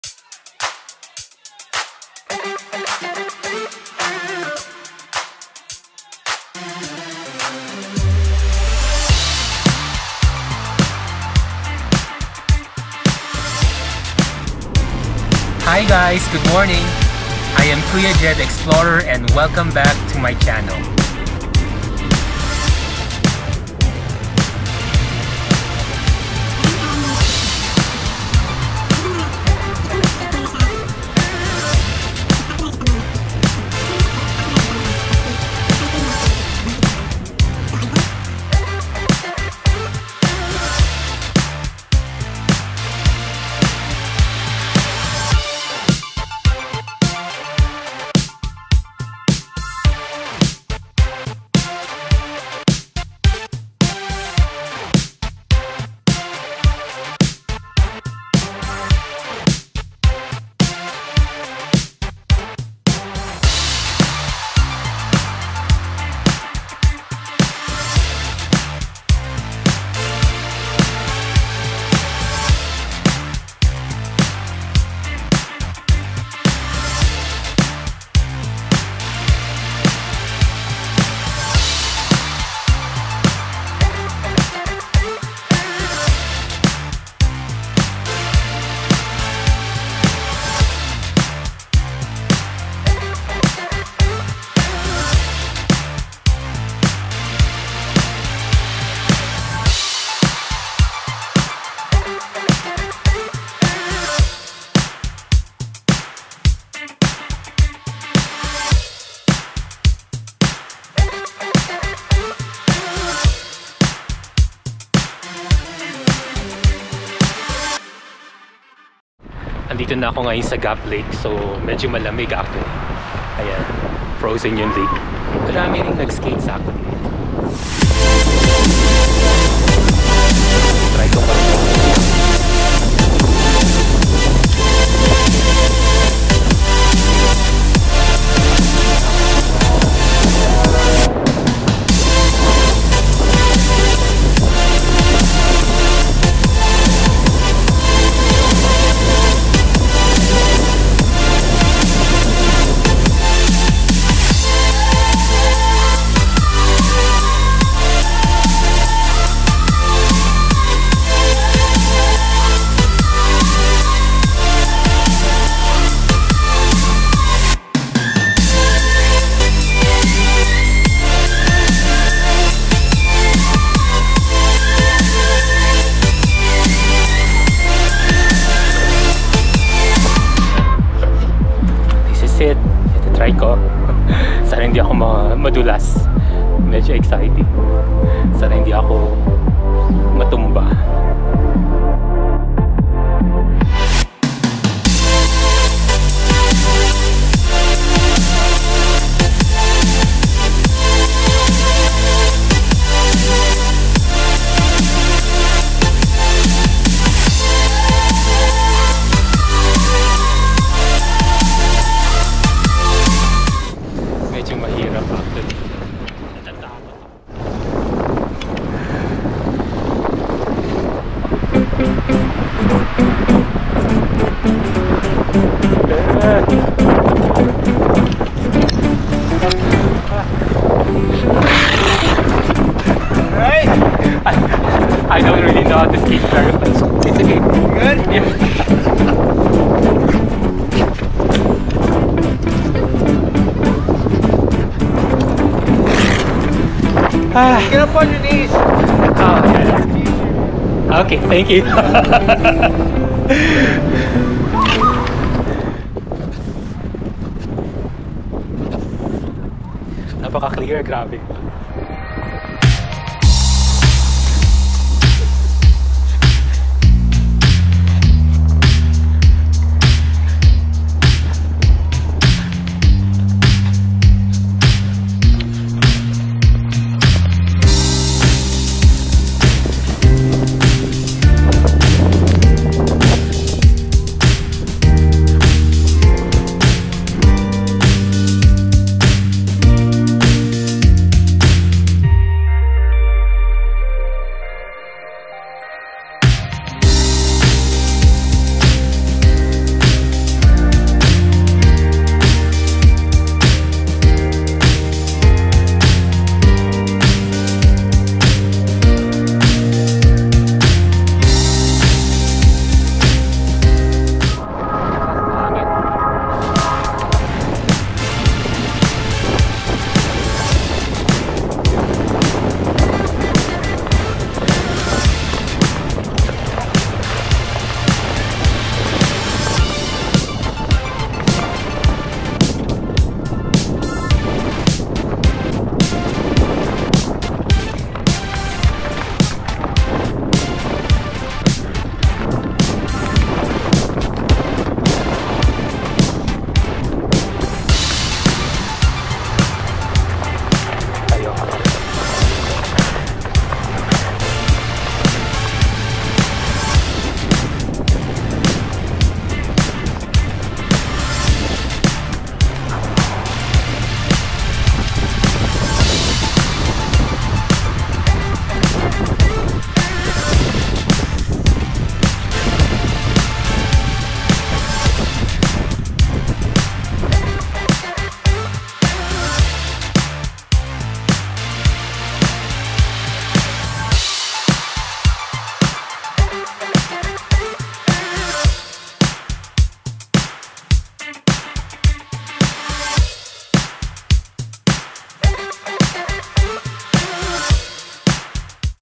Skating on Thin Ice | Frozen Gap Lake, Alberta, Canada
Skating on frozen lake is something to look forward to during winter. I started skating not too long ago and I am not good at it 😂. Gap lake is close to Calgary and many people come to skate in this beautiful lake. I hope you enjoy the scenery and excuse for me for my skating abilities 😂.

Video shot: Gopro hero 9
Video Edit: Final Cut Pro X